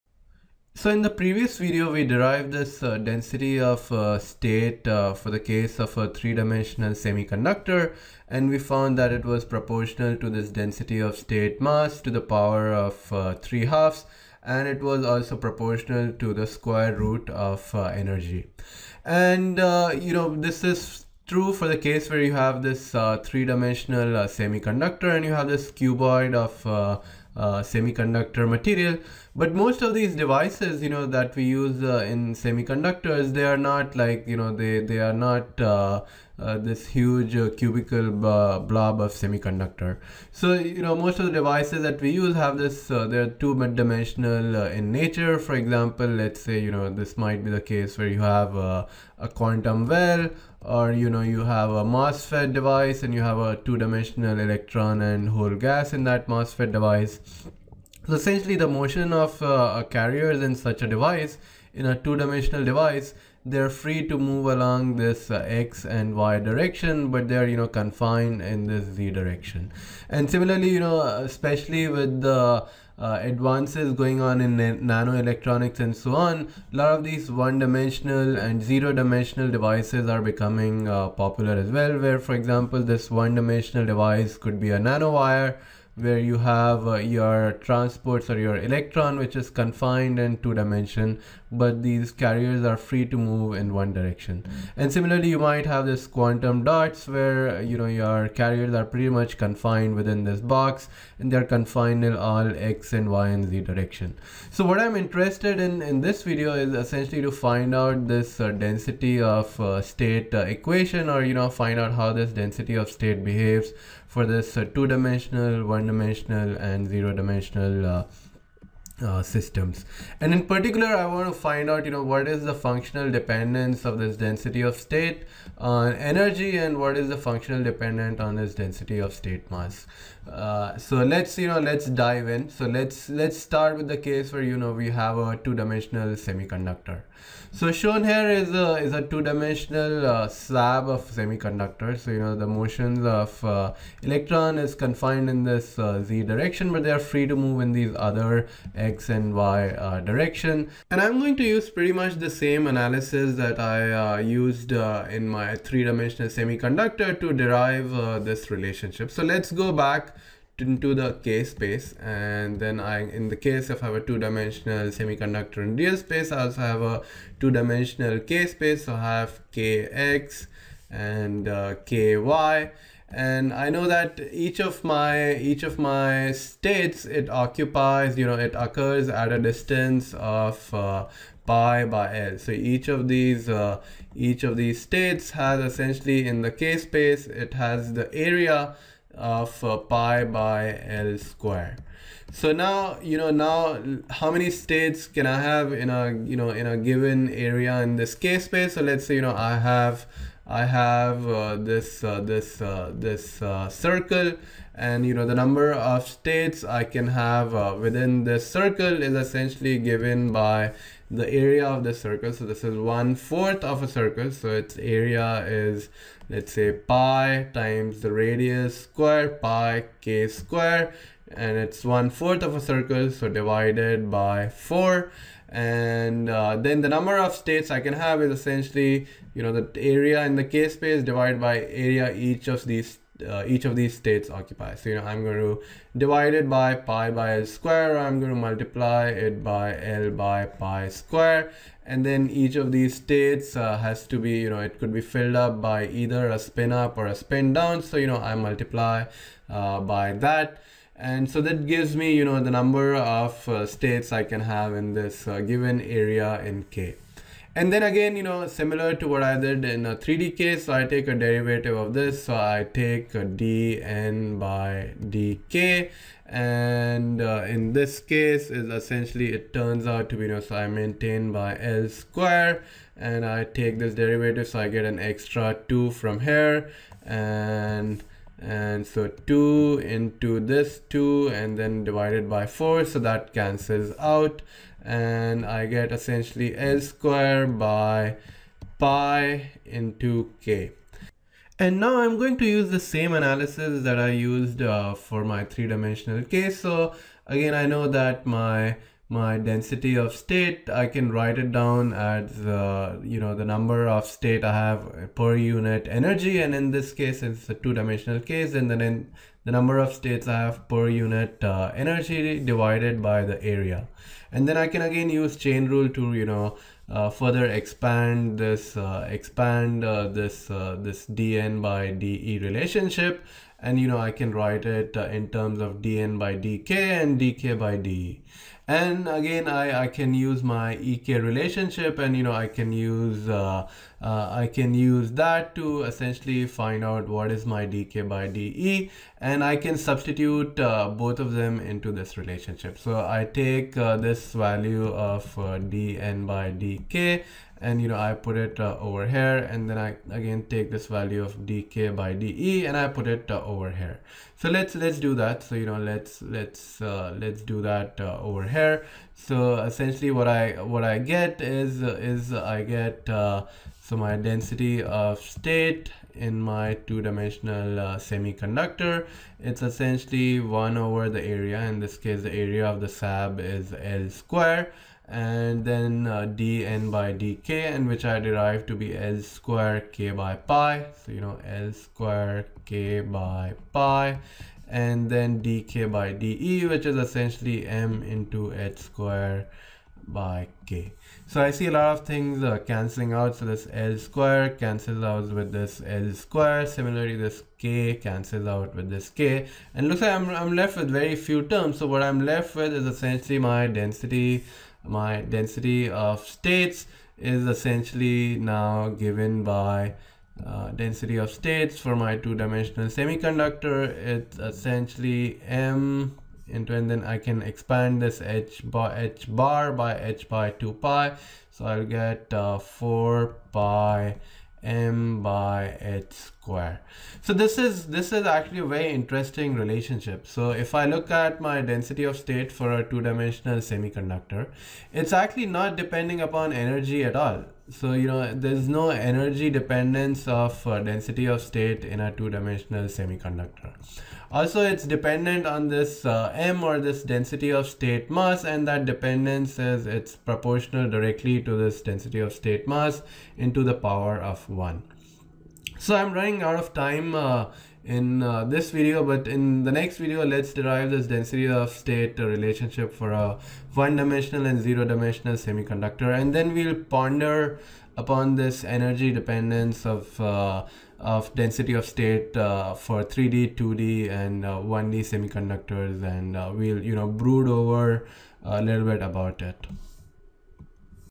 Density of States in a 2D, 1D, 0D semiconductor: part 1
A simple derivation for density of states in 2D, 1D, 0D semiconductor